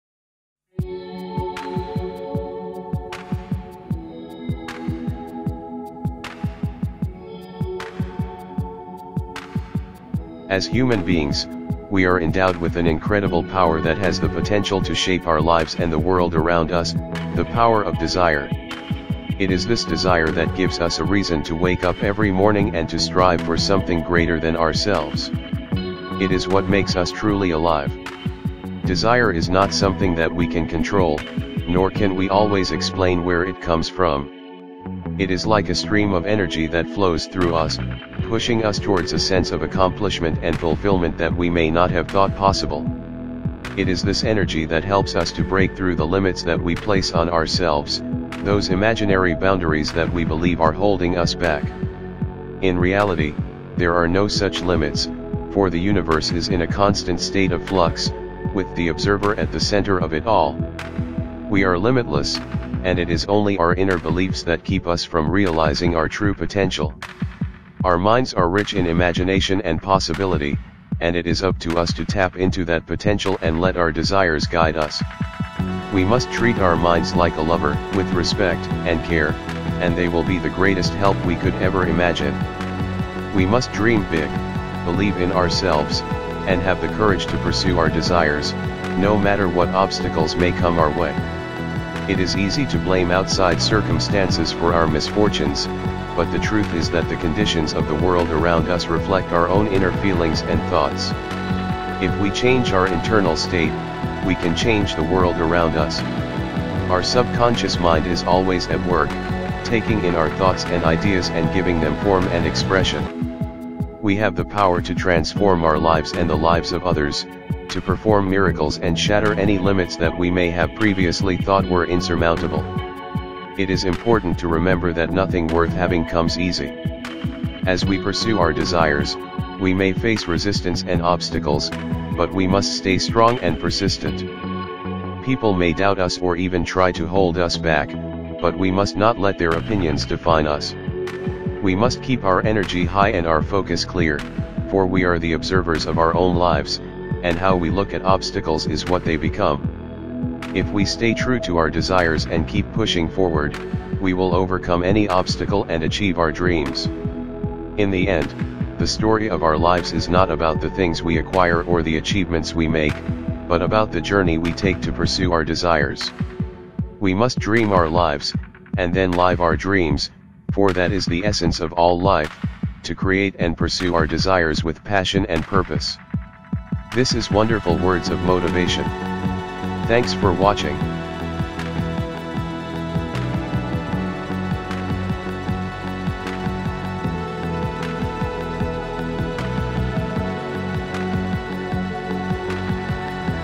The Power of Desire: Conquering Limits and Achieving Dreams
In this inspiring story, we explore the importance of having a desire in life and how it can push us to become truly alive. We learn that our mind is rich in imagination and possibilities, and by transcending our limitations, we can achieve anything we set our minds to. With a powerful message about the relationship between our inner beliefs and the outside world, this story encourages us to dream big and live our lives to the fullest. Join us on a journey of self-discovery and empowerment, and learn how to conquer yourself and conquer the world.